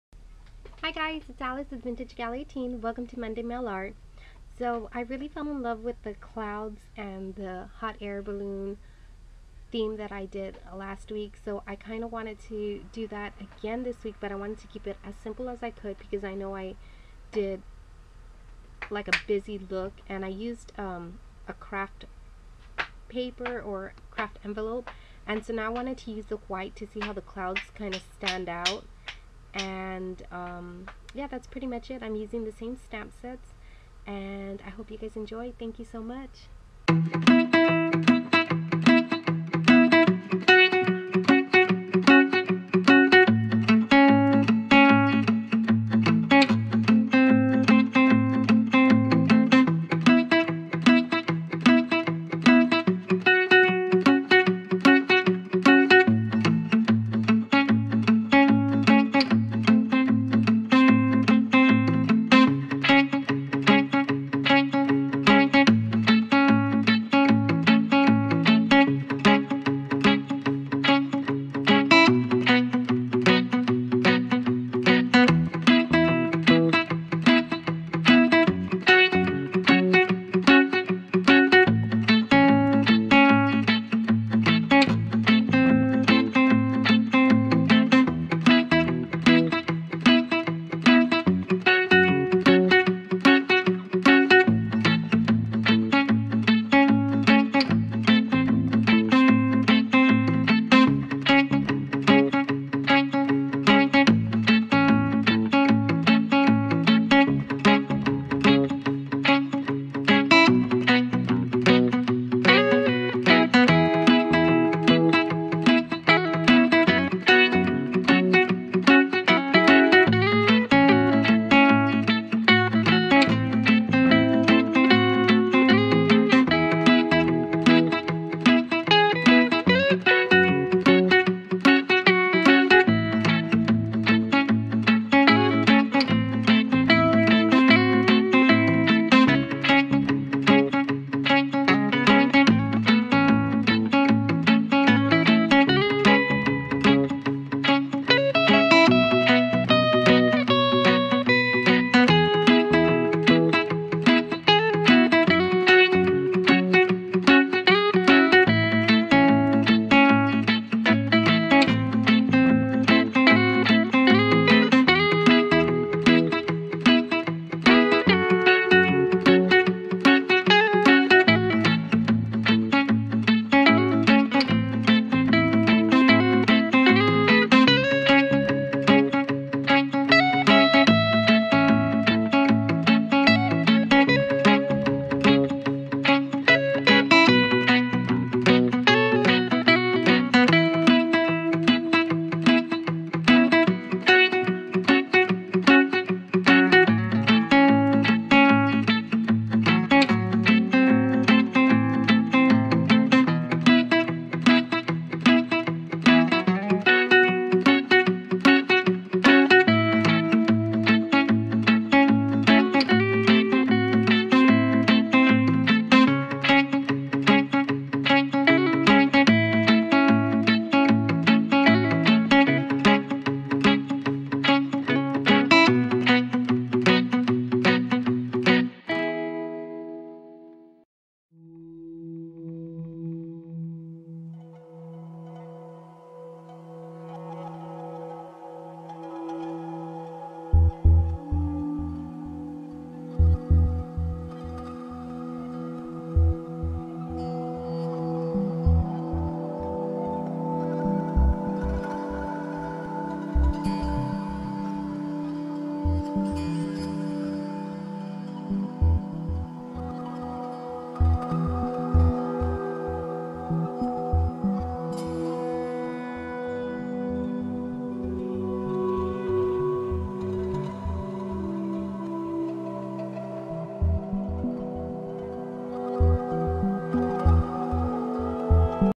Monday Mail Art June 30
Hi you all!
Welcome to Monday Mail Art
This will be another Butterfly Theme Envelope using the same stamp sets I used last week!
The Stamps of Life: Hot4Balloons
Stampin UP: Papillon Potpourri
The Greeting Farm: Clouds